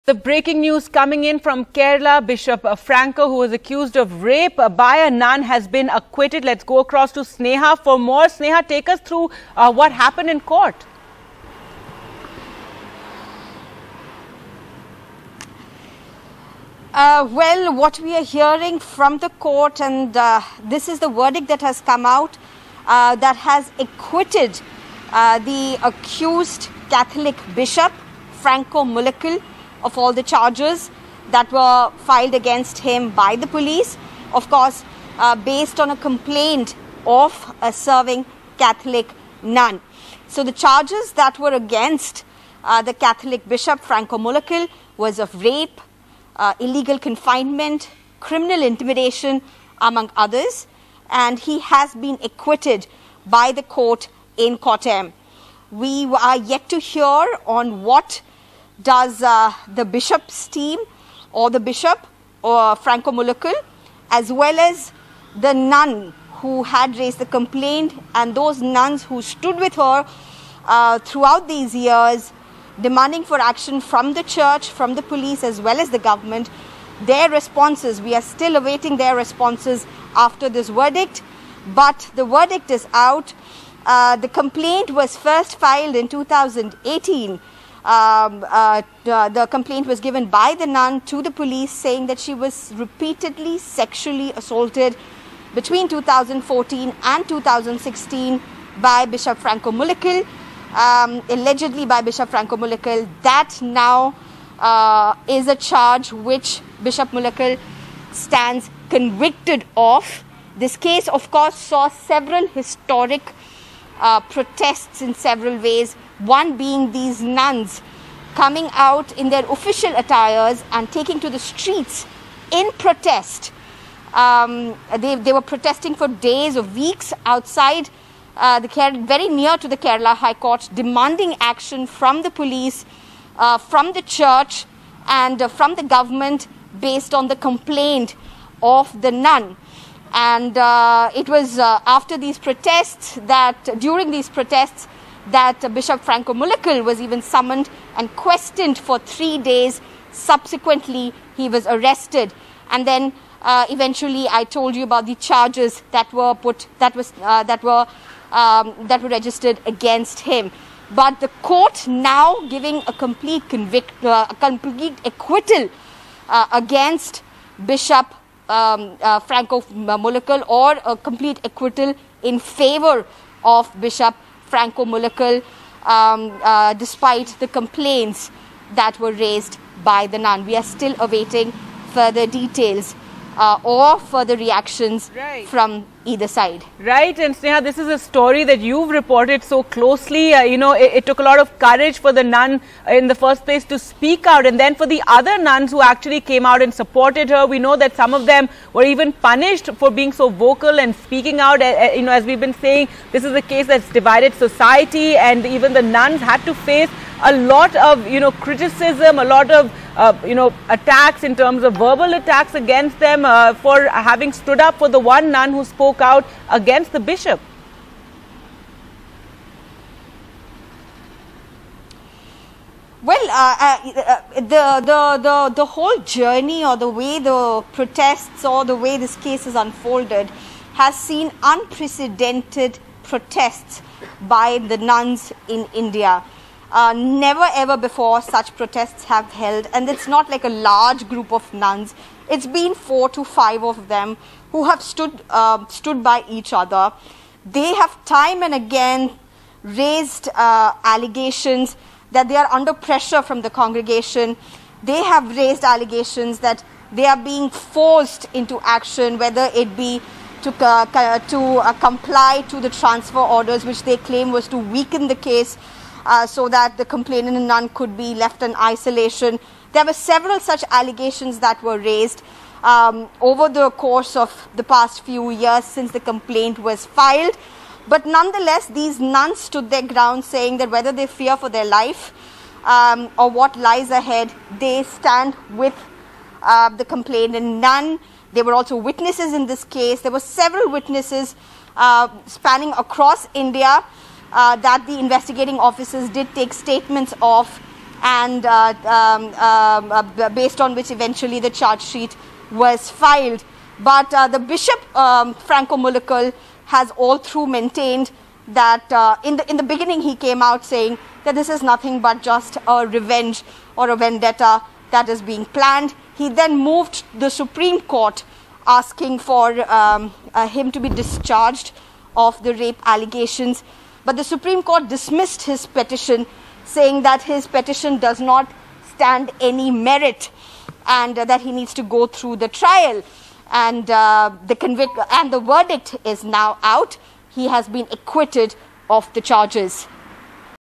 Bishop Franco Mulakkal, Accused Of Nun's Rape In Kerala, Acquitted
Bishop Franco Mulakkal, accused of raping a nun several times between 2014 and 2016, has been acquitted by a court in Kerala.
Franco Mulakkal, 57, was the first Catholic bishop in India to be arrested for assaulting a nun.